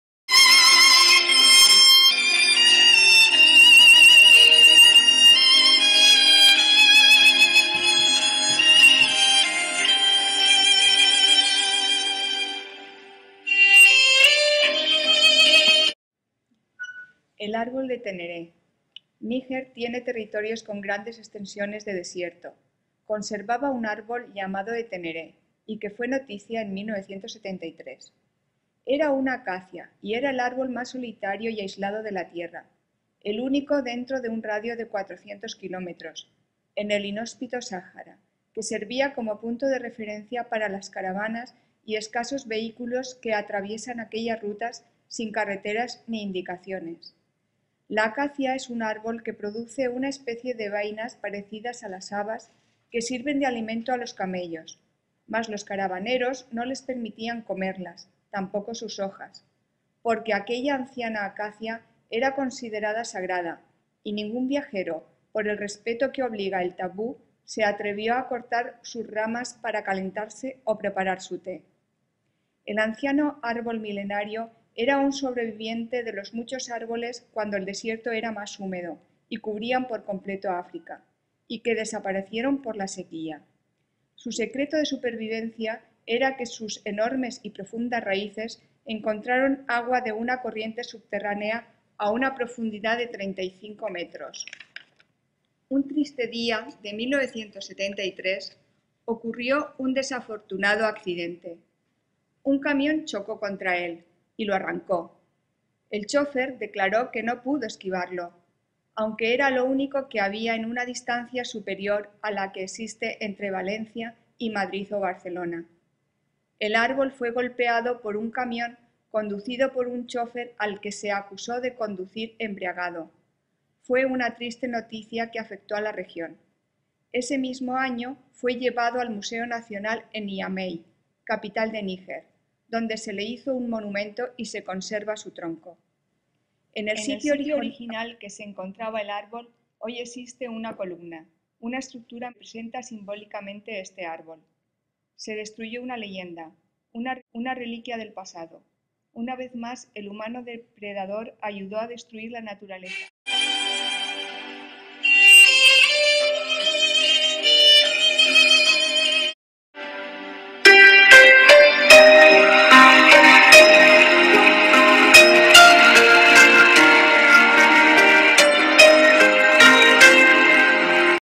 EL ÁRBOL SAGRADO QUE VIVIÓ EN EL DESIERTO DEL SAHARA
COMO SOBREVIVIÓ CIENTOS DE AÑOS EN EL SAHARA